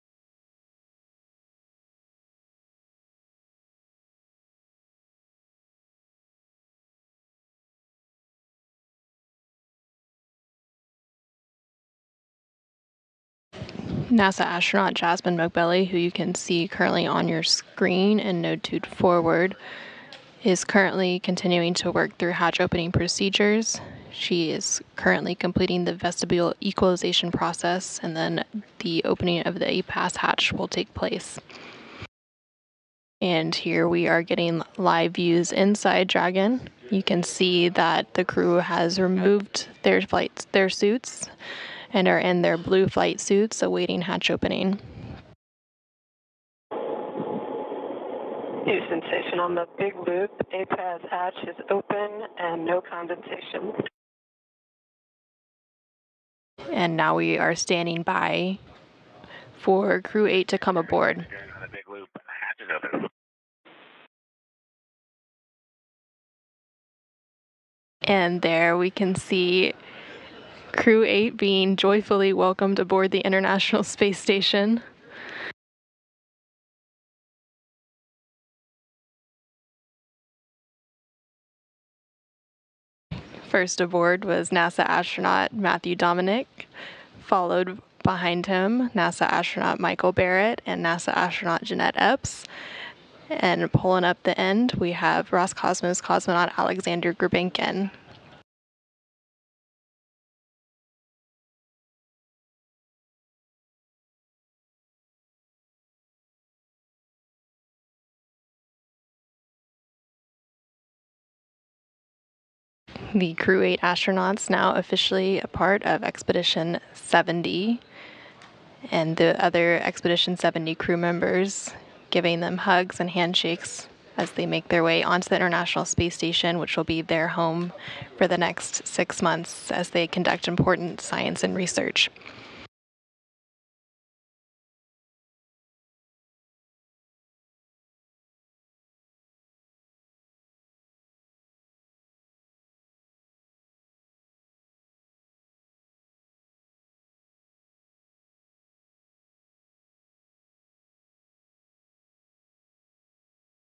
SpaceX Crew-8 hatch opening
The hatch of SpaceX’s Crew-8 “Endeavour” Crew Dragon spacecraft, with NASA astronauts Matthew Dominick, Michael Barratt, Jeanette Epps and Roscosmos cosmonaut Alexander Grebenkin, was opened on 5 March 2024, at 08:50 UTC (03:50 EST). Crew-8 is SpaceX’s eight operational mission for NASA’s Commercial Crew Program.
Credits: NASA/SpaceX
“Endeavour” Crew Dragon hatch opening